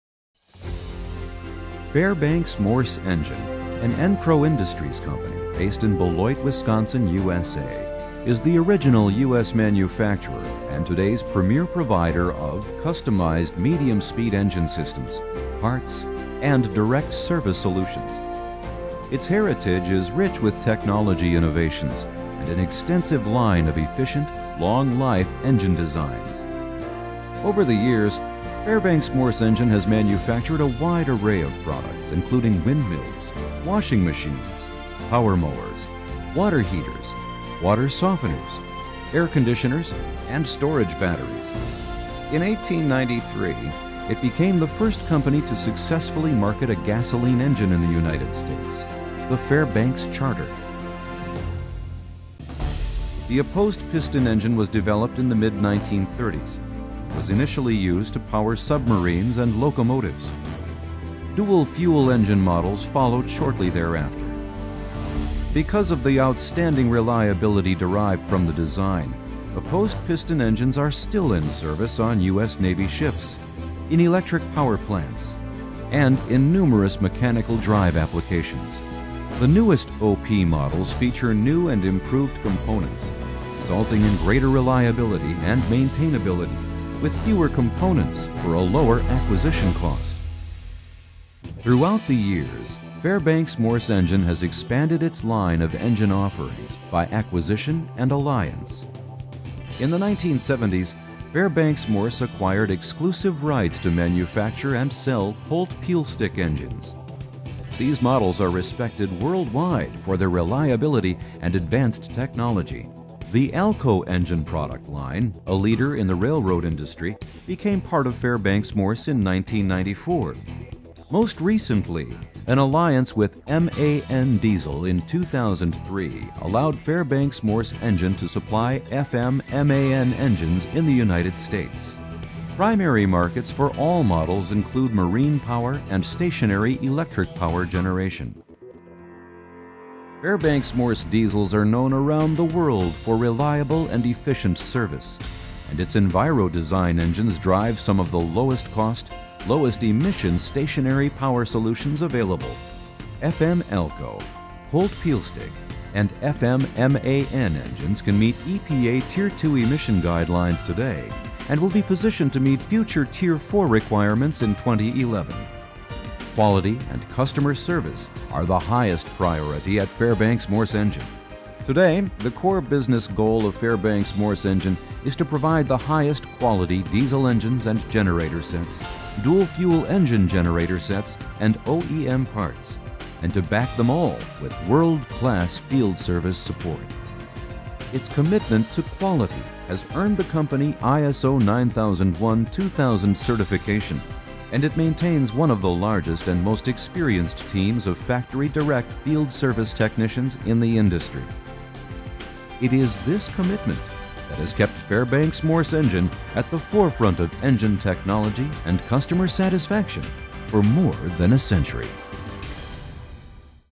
The History of Fairbanks Morse Engine
History of Fairbanks Morse Engine

Today, Fairbanks Morse Engine is the premiere manufacturer of innovative diesel and dual-fuel engines for marine and stationary power applications. Fairbanks Morse Engine's environmentally friendly Enviro-Design®, FM/ALCO, Colt-Pielstick, and FM MAN engines provide excellent fuel consumption with low emissions and are positioned to meet future emissions requirements in the coming years.